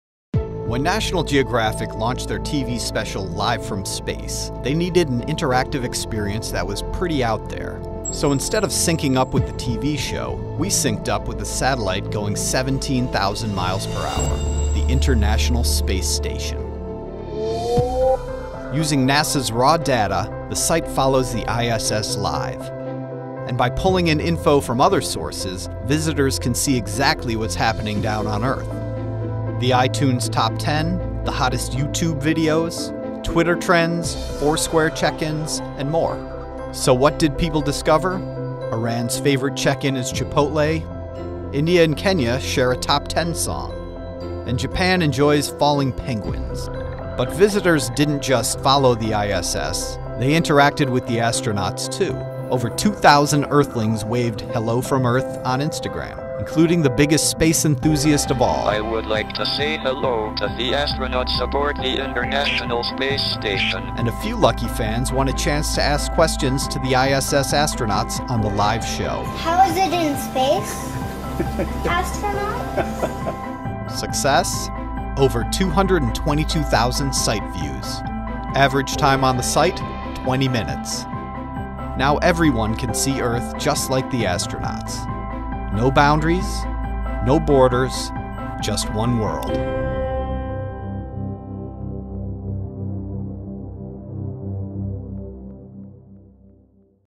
National Geographic - Live From Space
What happens when you sync up with a satellite going 17,000 mph around the Earth? You see the world from a whole new perspective. For National Geographic Channel’s TV special “Live From Space,” Mullen Lowe US created an interactive experience unlike any other.

LiveFromSpace.com synced up with the International Space Station and allowed visitors to see exactly what was happening down on Earth. Visitors could explore the top iTunes songs, YouTube videos, Twitter trends, Foursquare check-ins and more in any country around the world. Explore a world with no boundaries and no borders – just like the astronauts.

We’re proud to announce that in the process of making space history, we also made Mullen Lowe Group history, earning us our very first Emmy for Social TV Experience. Out of this world.